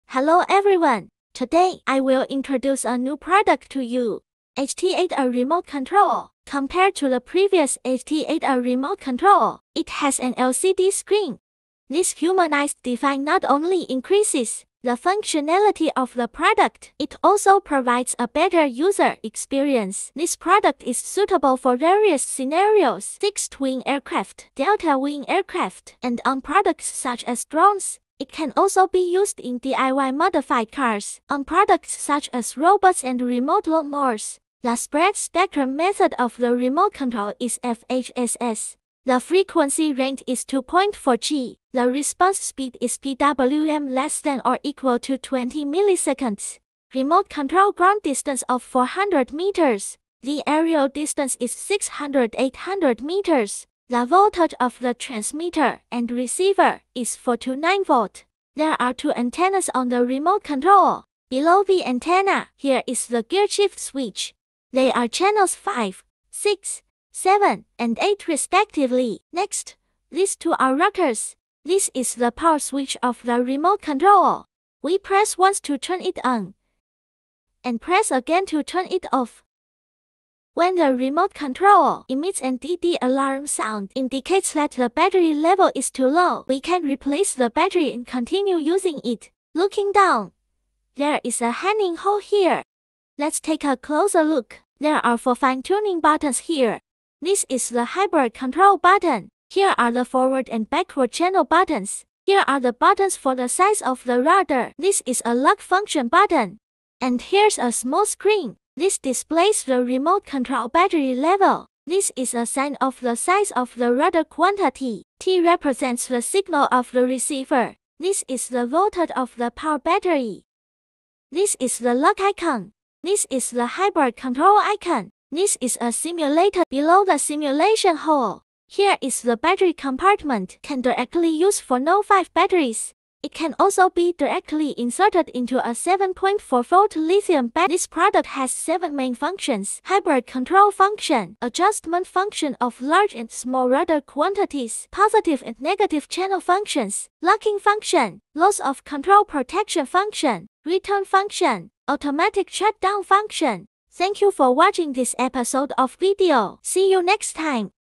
The new HT-8A remote control Nanny level product explanation.
The new HT-8A eight-way remote control comes with a small screen, which supports voltage return and signal return. It is suitable for model aircraft such as fixed-wing aircraft, delta-wing aircraft, and crossing aircraft, and can also be used for DIY modified cars, robots, and lawn mowers and other machine products.